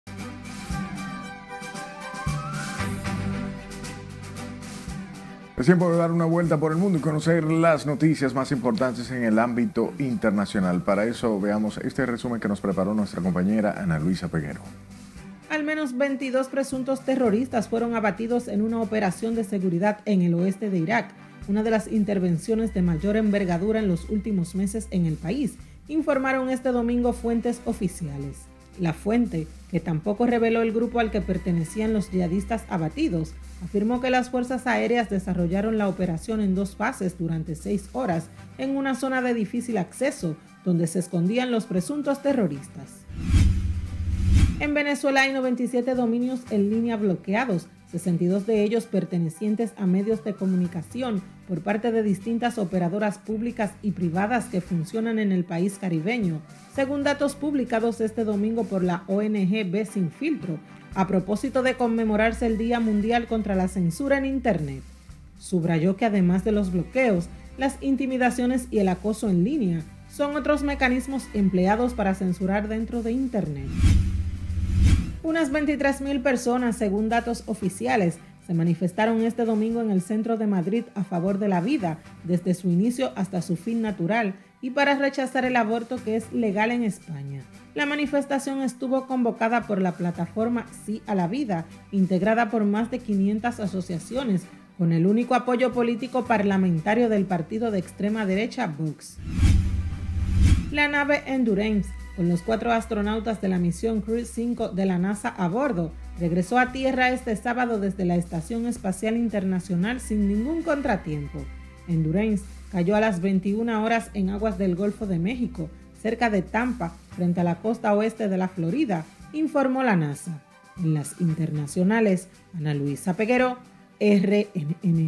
La guerra urbana engrosa la lista de desplazados en Haití
Es tiempo de dar una vuelta al mundo y conocer las noticias más importantes en el ámbito internacional… para eso veamos este resumen que nos preparó nuestra compañera @ana_peguero.